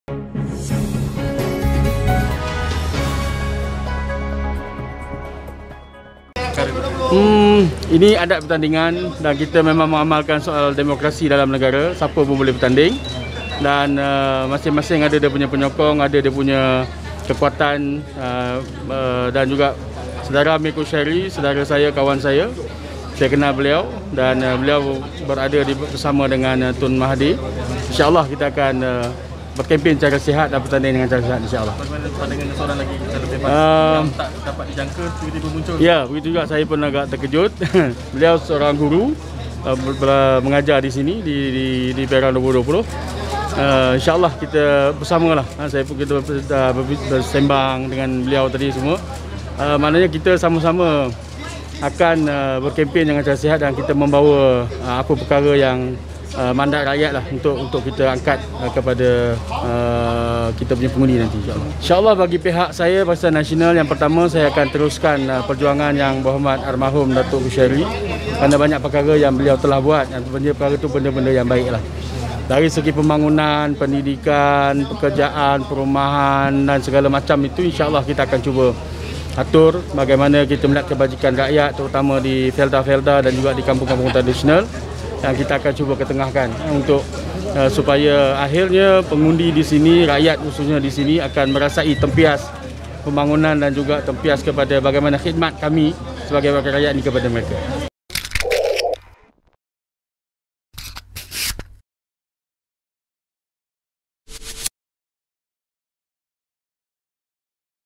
PRK SLIM : Saya Akan Teruskan Perjuangan Datuk Mohd Khusairi Abdul Talib
Calon Barisan Nasional (BN), Mohd Zaidi Aziz selepas menghantar borang pencalonan pada hari penamaan calon sempena Pilihan Raya Kecil (PRK) N. 58 di Dewan Sri Tanjung, Majlis Daerah Tanjung Malim, Perak.